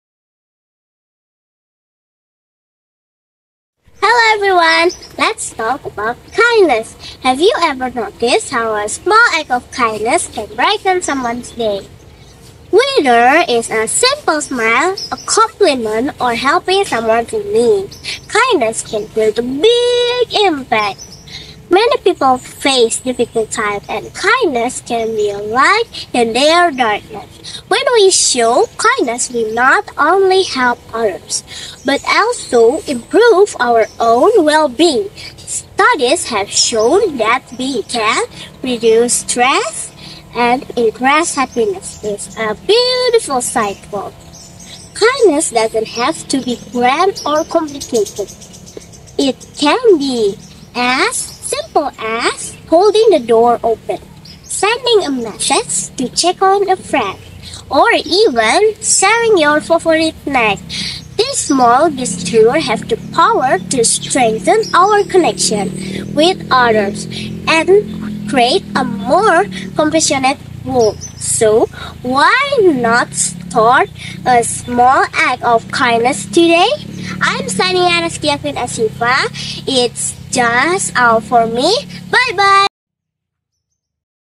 Lomba Story Telling Jenjang SD_SDS Bumitama Cempaga Hulu_Azkia Queen Asyifa_Kindness
Small Kindness like smile can helping everyone